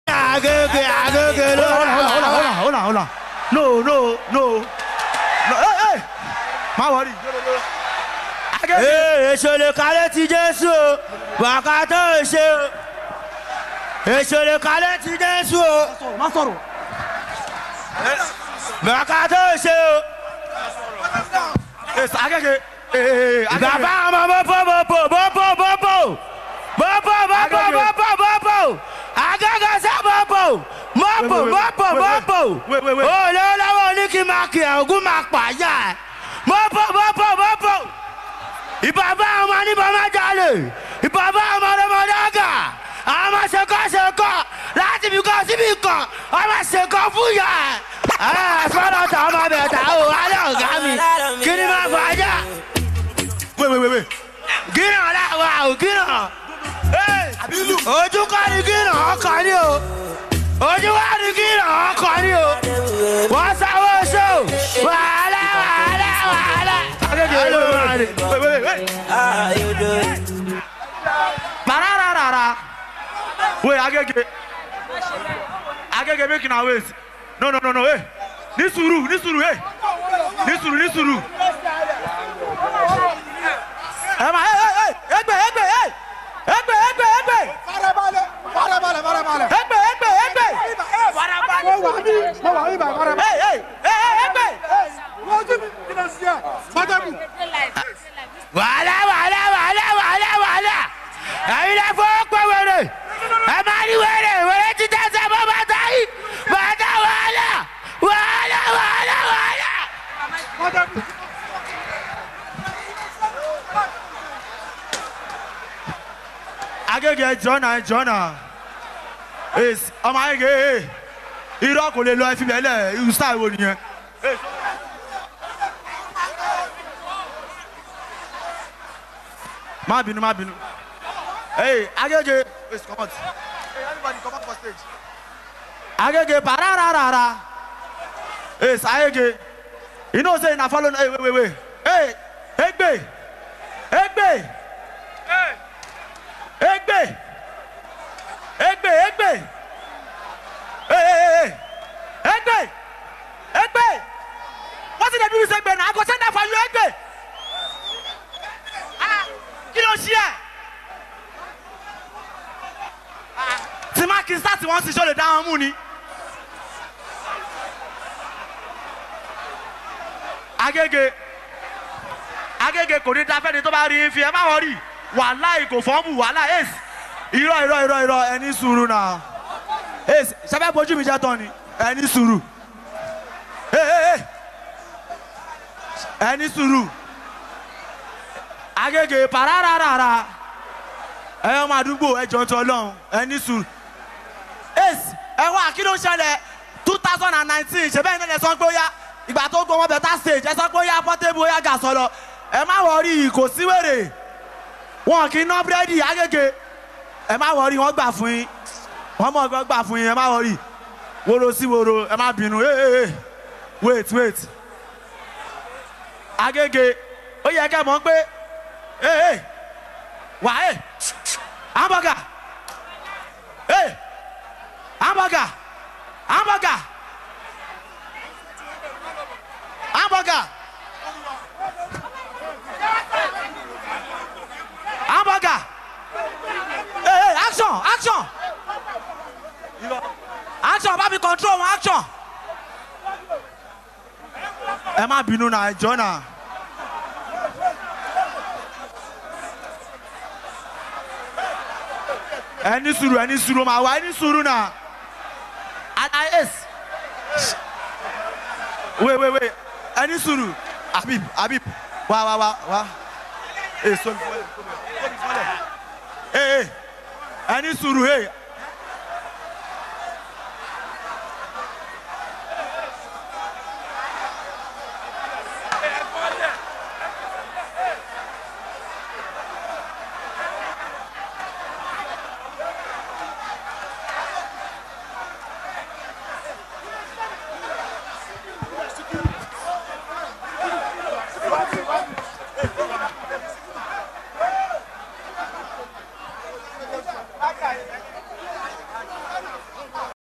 📍Portable vs Agege | he nearly collect | Small Doctor pleads on his behalf 👋
📍Portable vs Agege | he nearly collect 👋